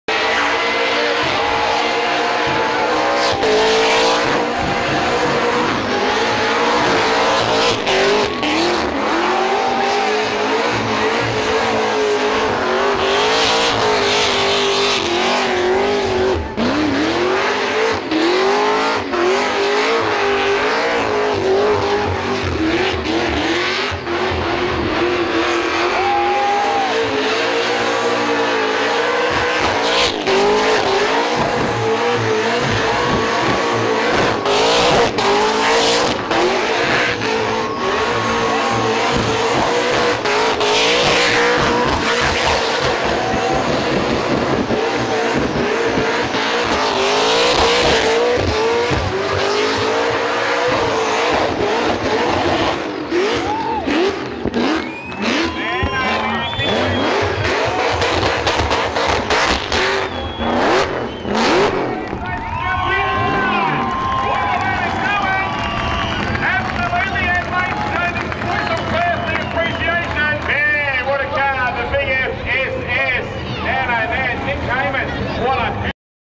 WIDE OPEN Pro Class Burnout Qualifying OZNATS Adelaide
Mick Hamon in WIDE OPEN with his Supercharged Blown HSV E Series Clubsport qualifying at OZNATS Adelaide Australian Performance Nationals burnout competition